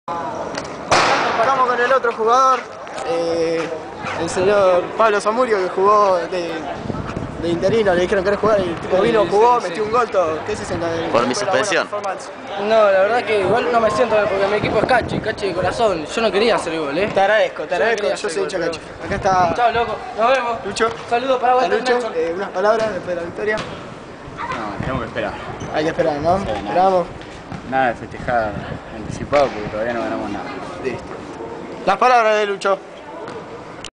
Fecha # 7 (declaraciones en caliente parte 2)
Siguen las declaraciones de los players tras el triunfo vs BR-2 q nos acerca cada vez más a la gloria.